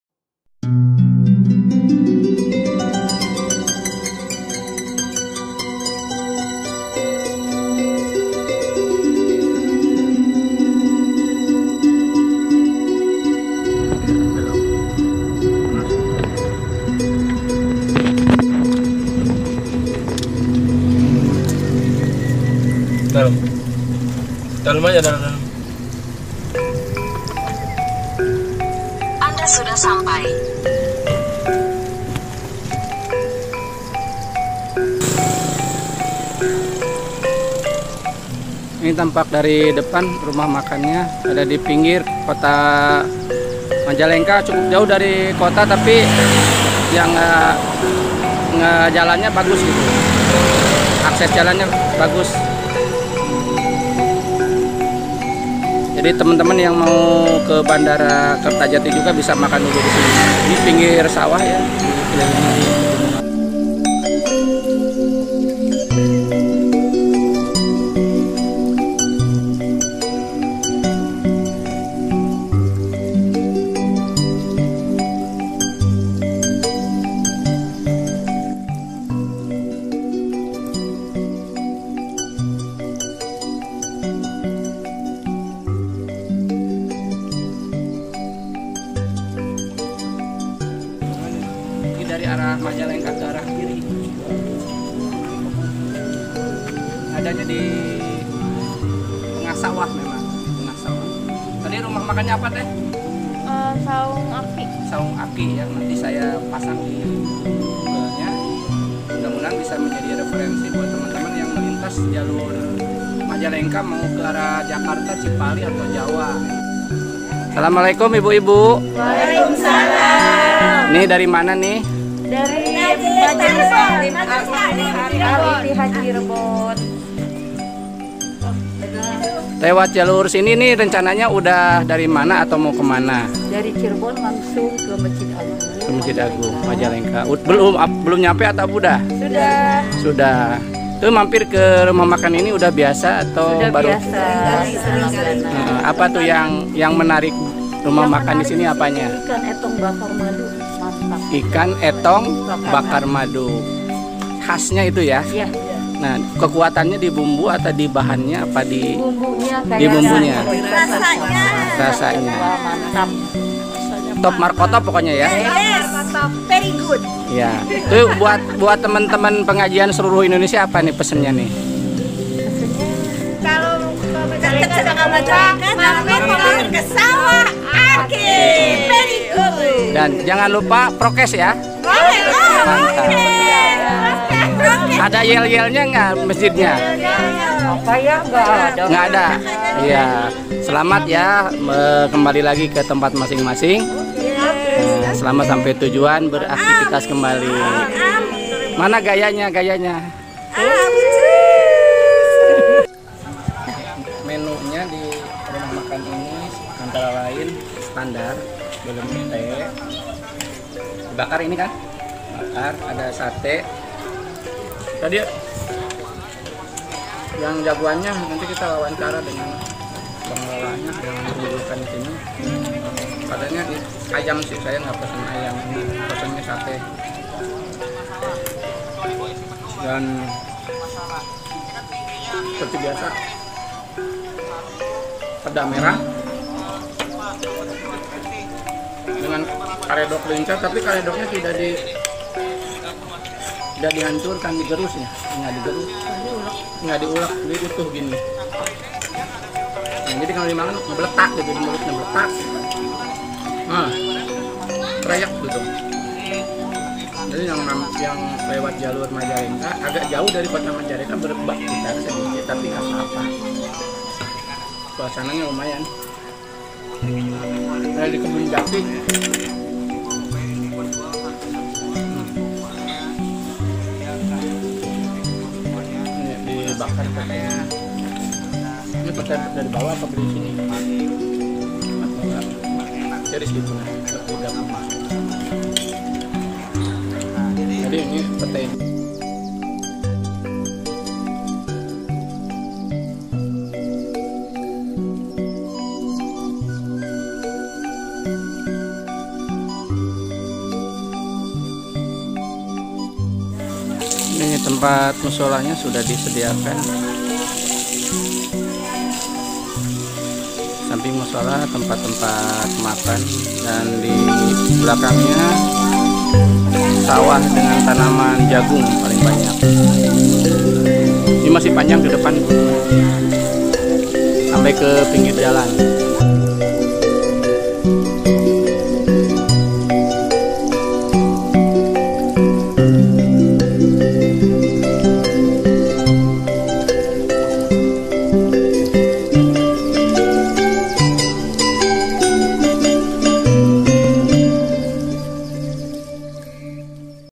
LELAH LAPAR TENGAH PERJALANAN, MAMPIR DI RM SAWAH AKI MAJALENGKA JABAR
Salah satu Rumah Makan yang bisa dijadikan alternatif, ketika lewat jalur Majalengka adalah sawah Aki, dengan tempat yang nyaman, parkir yang luas dan pemandangan pesawahan dan Ikan Bakar Entong Madu ...boleh di jajal